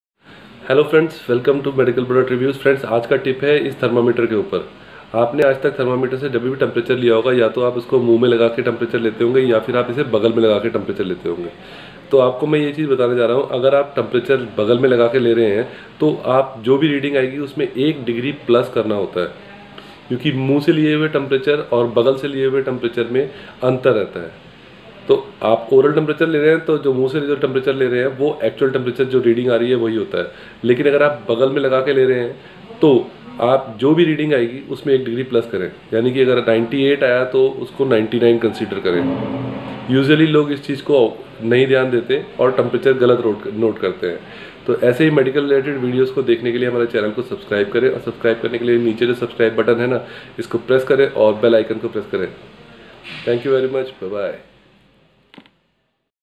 How to use thermometer accurately /क्या गलतियां करते हैं लोग थर्मामीटर इस्तेमाल करने में
इस वीडियो में मैंने बताने की कोशिश कि किस तरीके से थर्मामीटर से बुखार नापना चाहिए
आमतौर पर लोग बुखार 2 तरीके से नापते हैं या तो वह थर्मामीटर को मुंह में लगाकर बुखार नापते हैं या फिर बगल में
मुंह में लगाकर थर्मामीटर नापने से जो भी रीडिंग आती है वही मानी जाती है परंतु कुछ केस में थर्मामीटर को बगल में ही लगाकर टेंपरेचर को नापा जाता है जैसे कि छोटे बच्चे l
बगल में लगाने से थर्मामीटर se infection फैलने के चांसेस भी कम रहते
जब कभी भी थर्मामीटर को बगल में लगा कर देखें तब जो भी reading आए उसमें 1 डिग्री प्लस करना चाहिए l